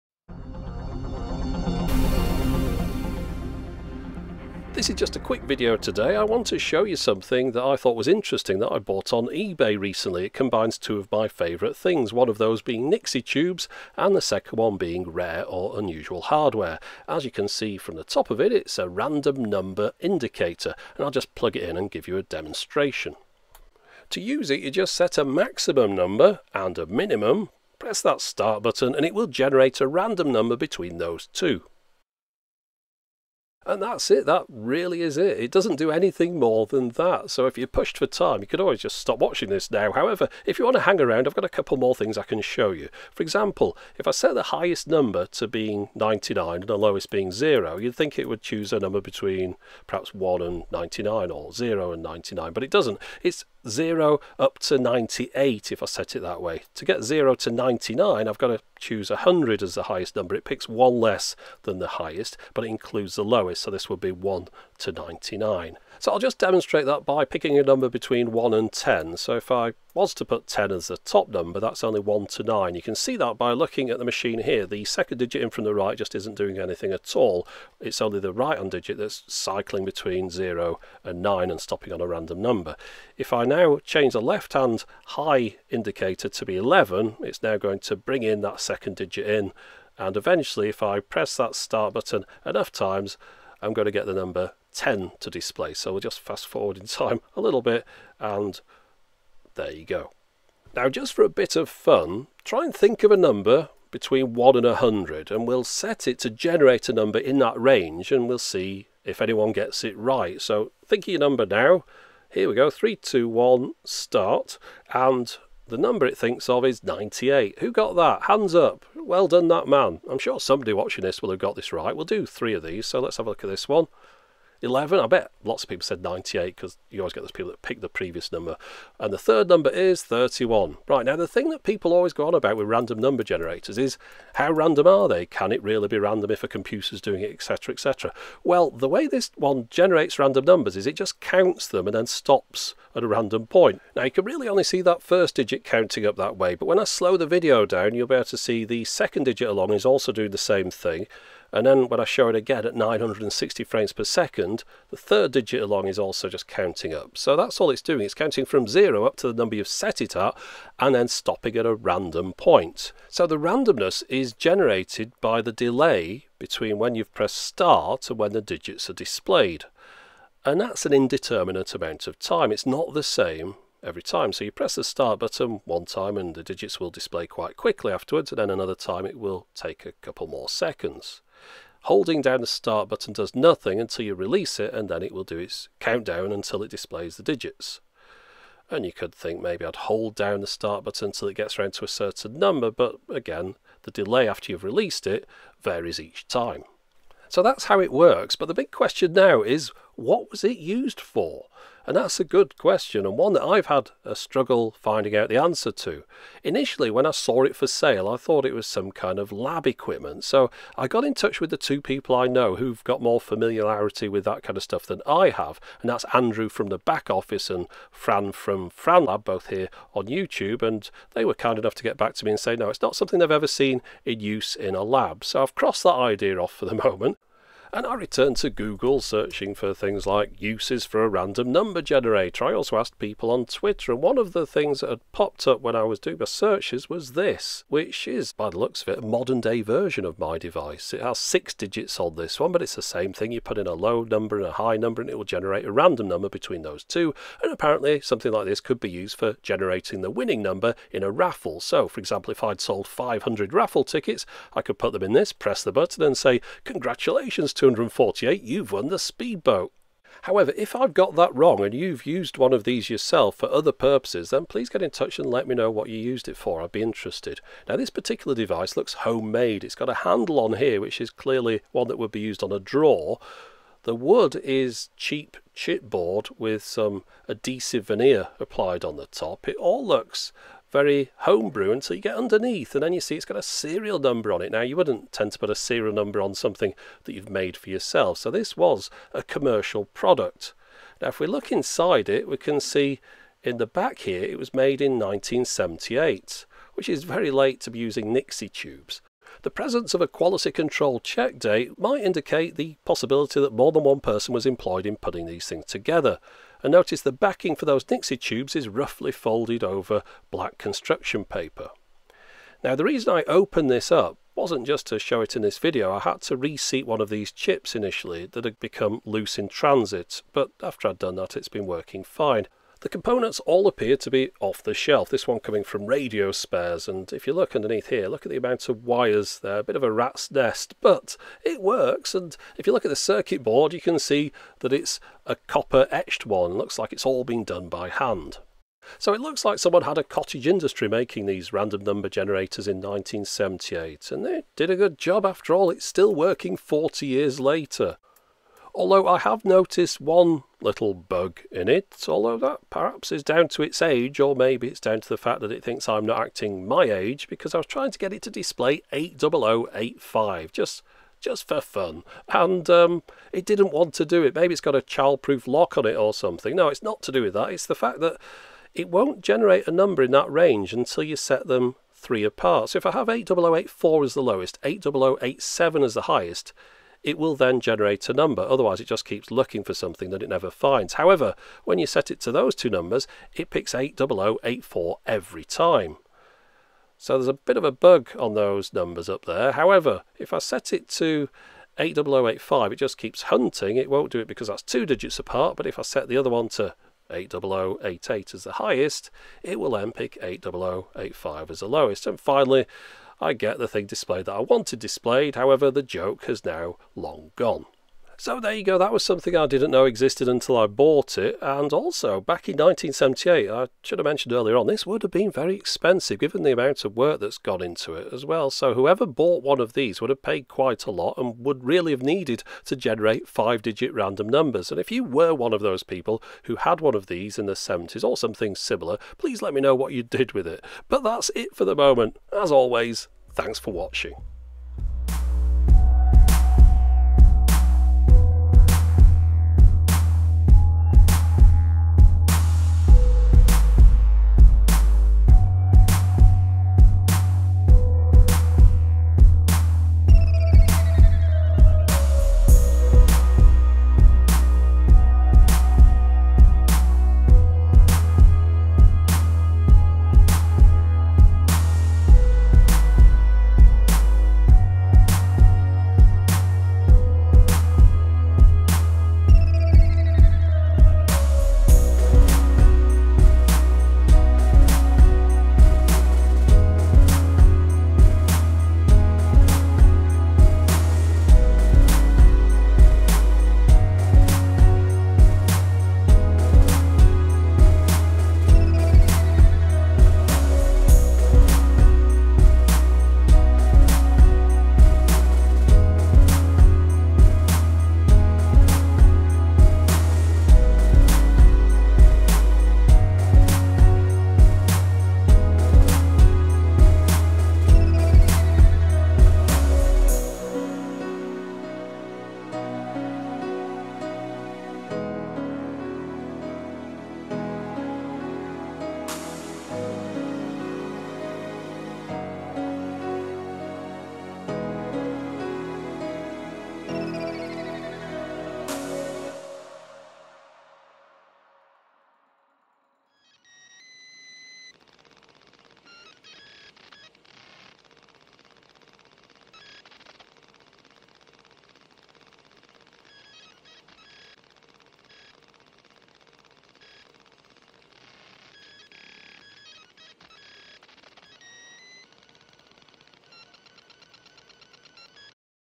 This is a bit random - Vintage Random Number Generator
An unusual piece of tech from the 1970s - a Random Number Generator.
The ebay advert for this suggested that the buyer could gut it for the nixie tubes. When I read that, I knew that I had to rescue this unusual little piece of electronics history.

SUPPORT
Patrons usually have early access to videos

Nu-spin 1978